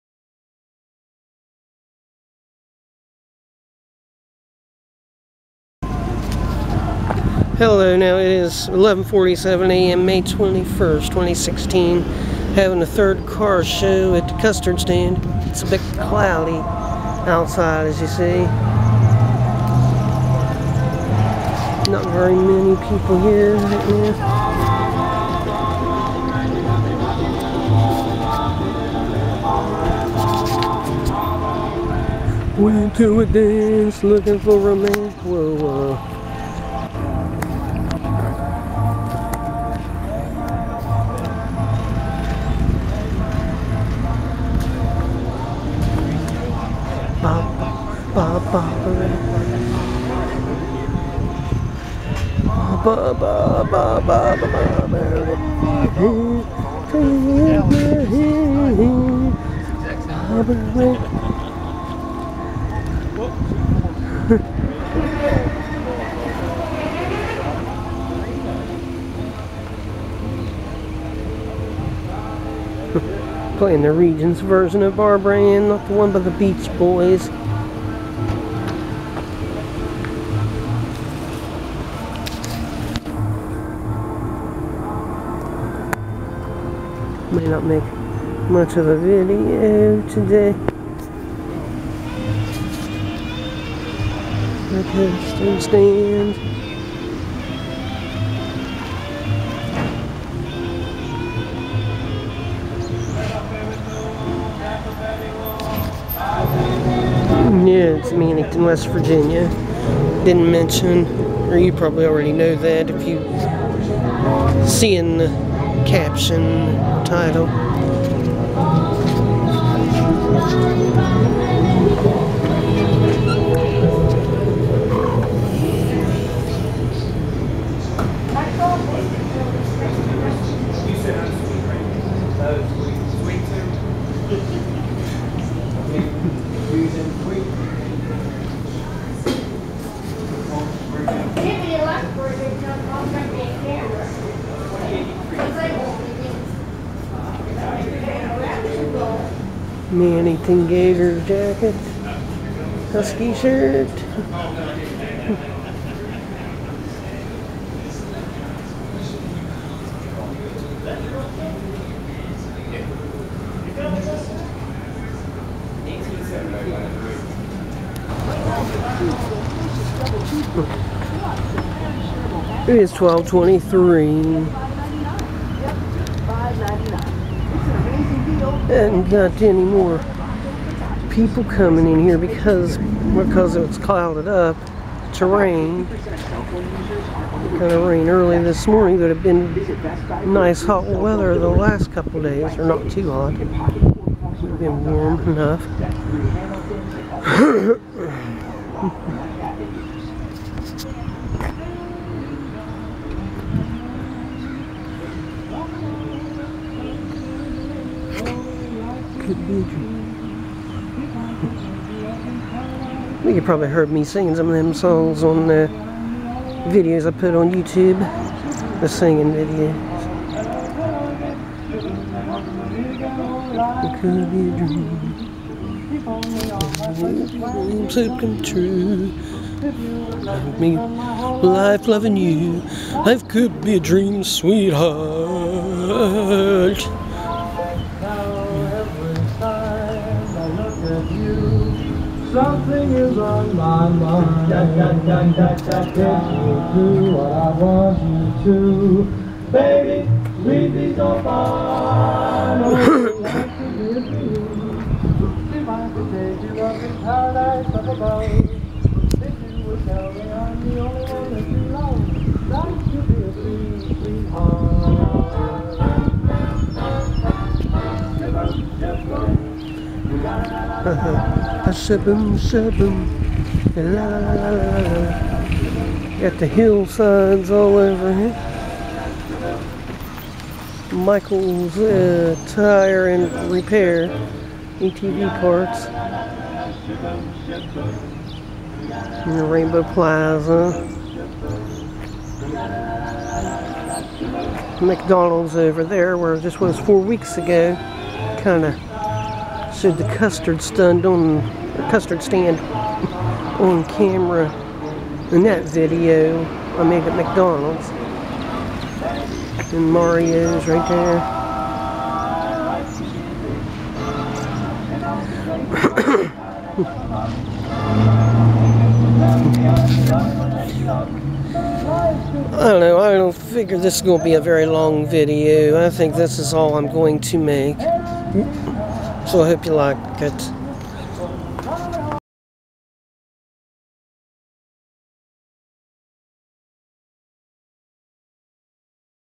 Mannington Custard Stand Car Show-May 21, 2016
Video of the third Car Show at the Custard Stand in Mannington, West Virginia.  There wasn't a big turn-out.  It was cloudy most of the morning and rained some that afternoon.